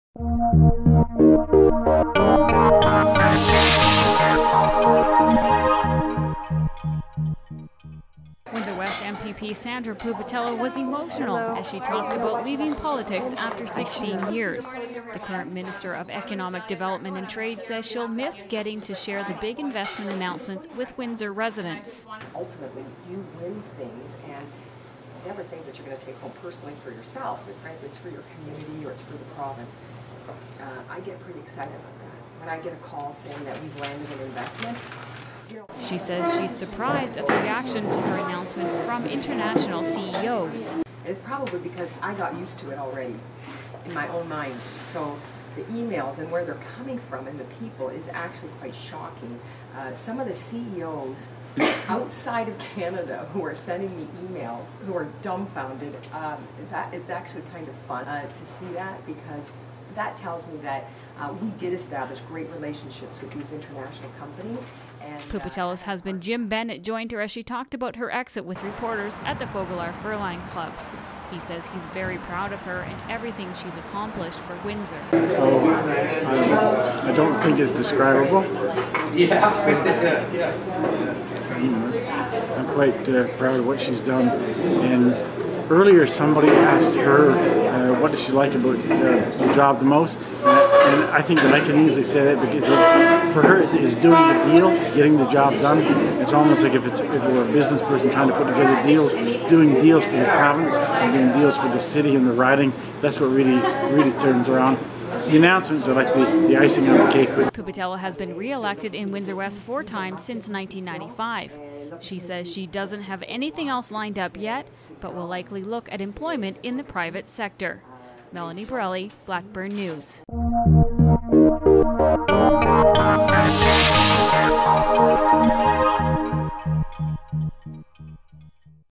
Windsor West MPP Sandra Pupatello Leaving Politics
Windsor West MPP Sandra Pupatello announces she won't be running in the
provincial election this fall.